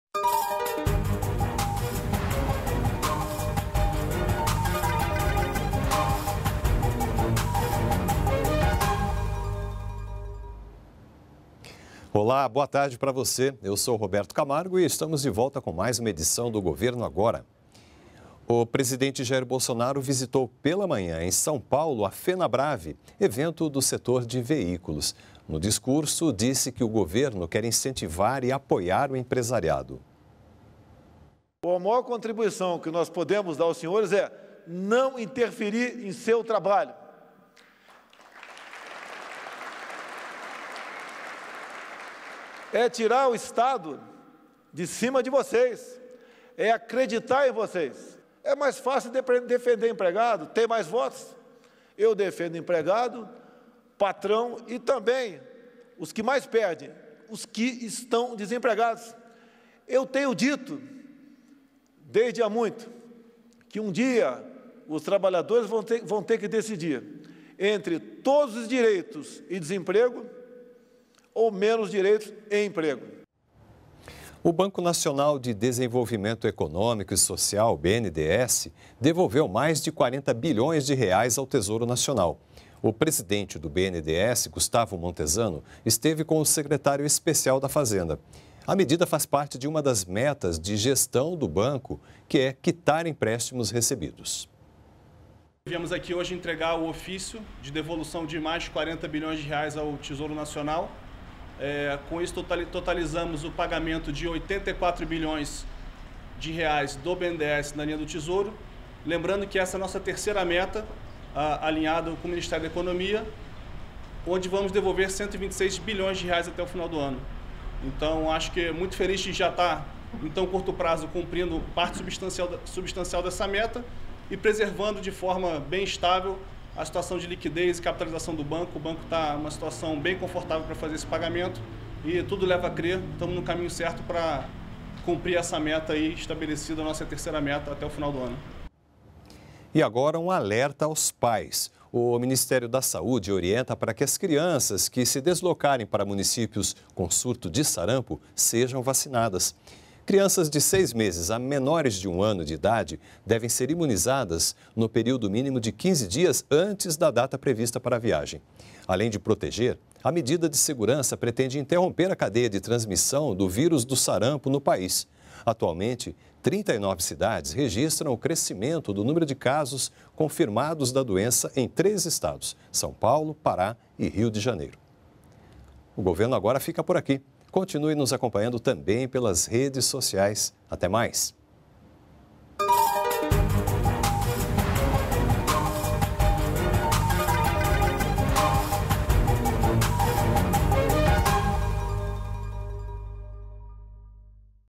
Bolsonaro visita , em São Paulo, a Fenabrave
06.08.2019 - O presidente Bolsonaro visitou pela manhã, em São Paulo, a Fenabrave.O BNDES devolveu mais de R$40 bilhões ao tesouro nacional. O Ministério da Saúde orienta que crianças que se desloquem para municípios com surto de Sarampo se vacinem.